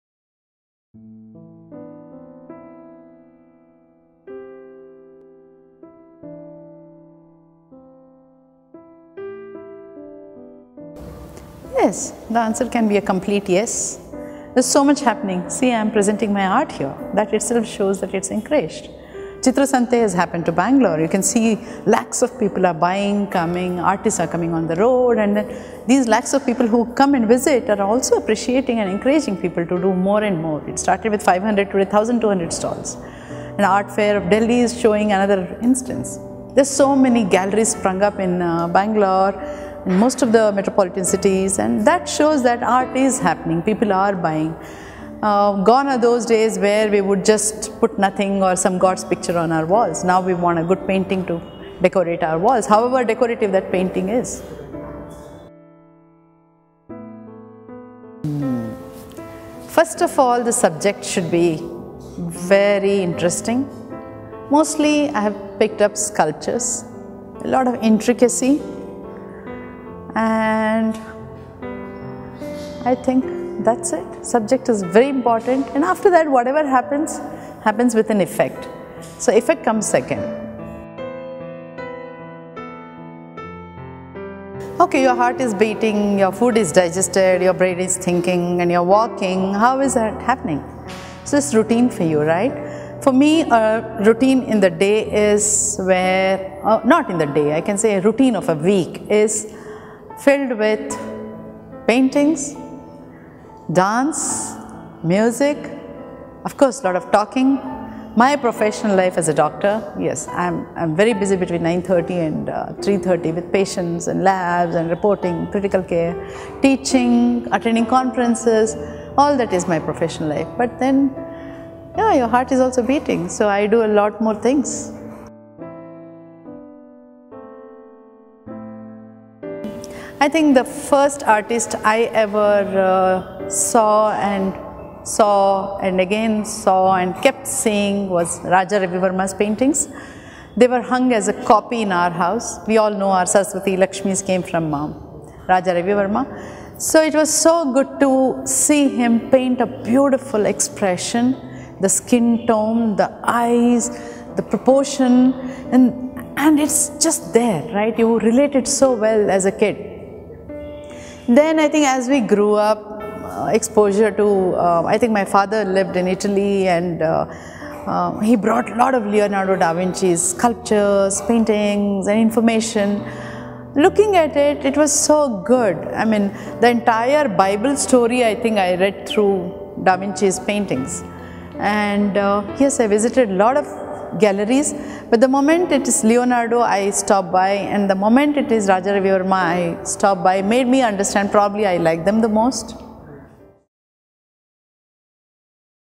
Artist Talk 1: Seemanthini Desai - Revisited | Gallery Manora
In this Artist Talk - CandidWithSeemanthini, Dr. Seemanthini Desai talks to us about her passion in the arts, what inspires her and her ongoing love affair with her subjects.
GalleryManora's ongoing exhibition Revisited showcases Seemanthini Desai's oil paintings of sculptures found in the AjantaCaves. Her canvases evoke a surreal calm in the viewer, reminding us of the beauty of sunsets and stone.
11 am - 7 pm | Monday - Saturday | 04 March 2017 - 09 April 2017